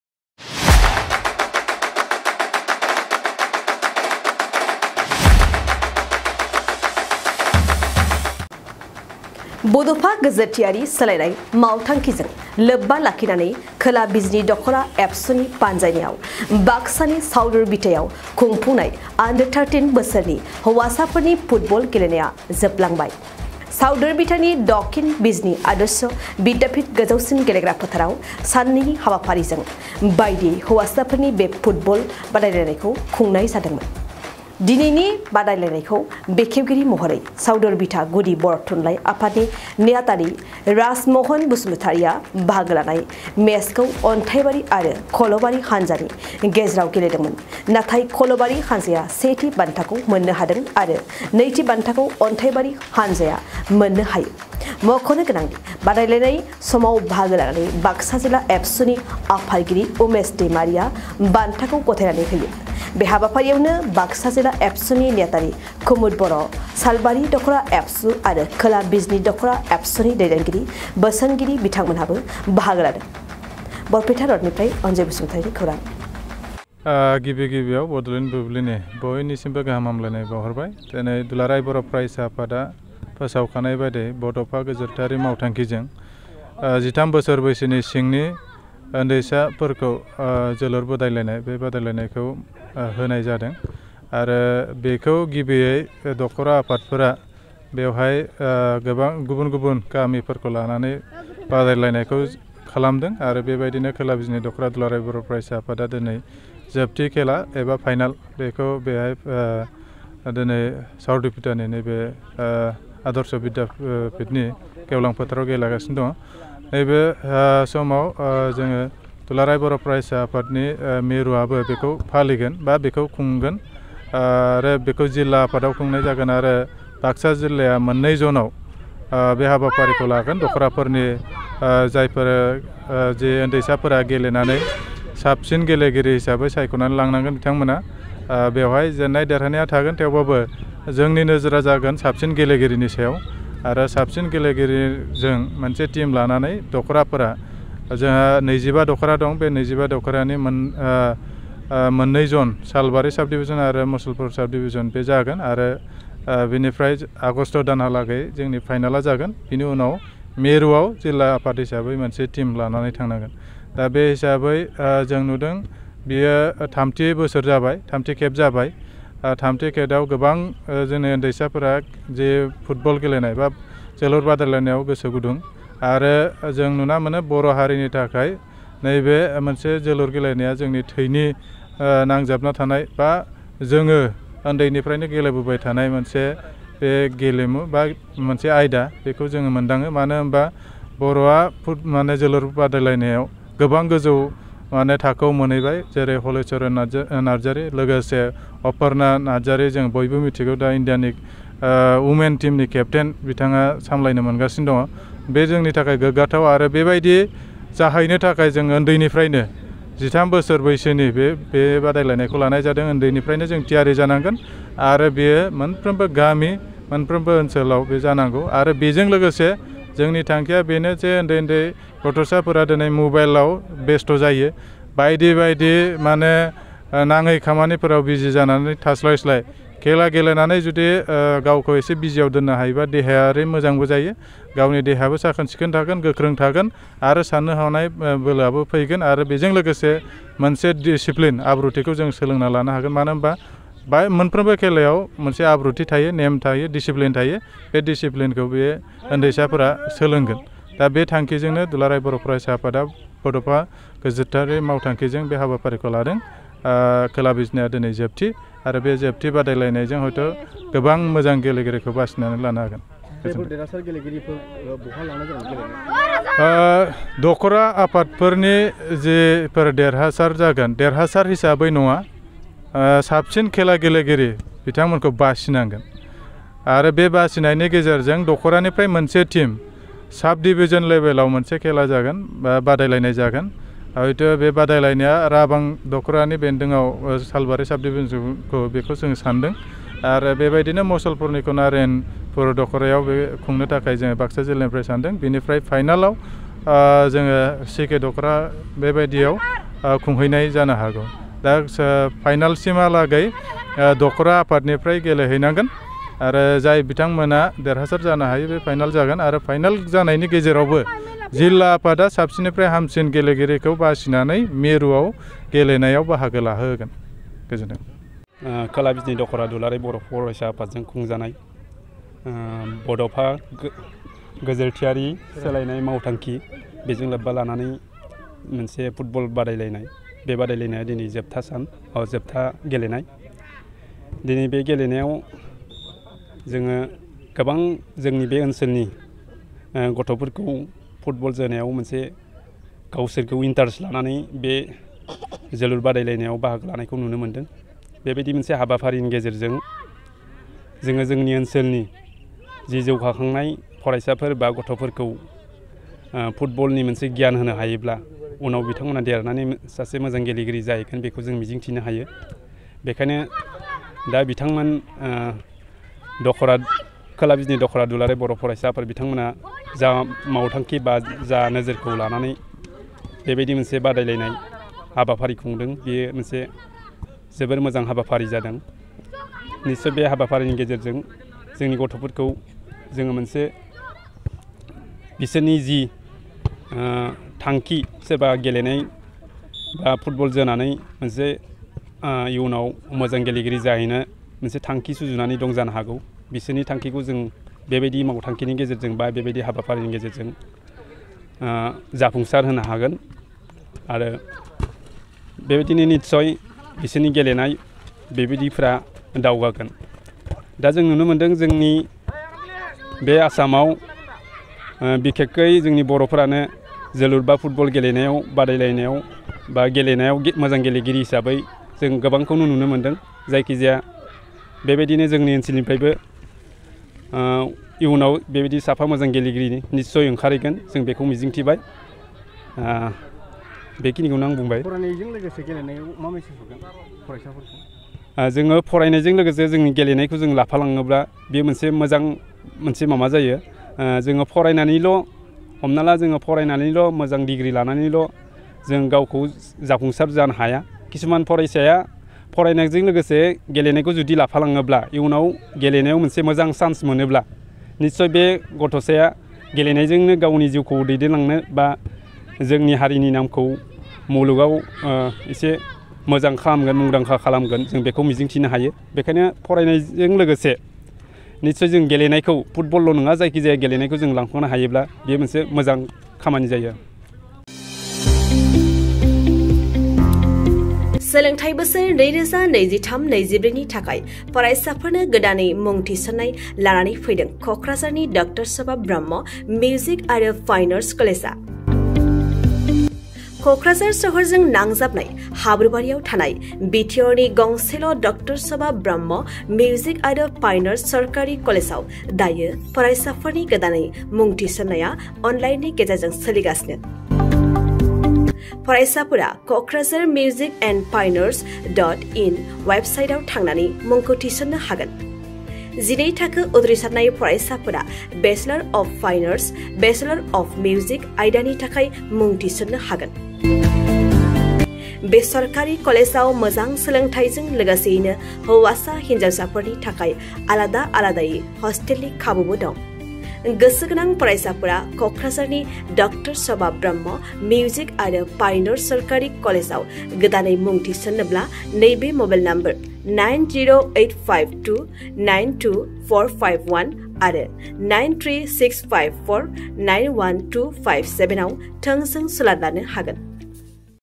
बड'फा गोजोरथियारि सोलायनाय मावथांखिजों लोब्बा लाखिनानै खोला बिजनी दख'रा एबसुनि बान्जायनायाव बा
बड'फा गोजोरथियारि सोलायनाय मावथांखिजों लोब्बा लाखिनानै खोला बिजनी दख'रा एबसुनि बान्जायनायाव बाक्सानि साउदरभिठायाव खुंफुंनाय आन्डार-13 बोसोरनि हौवासाफोरनि फुटबल गेलेनाया जोबलांबाय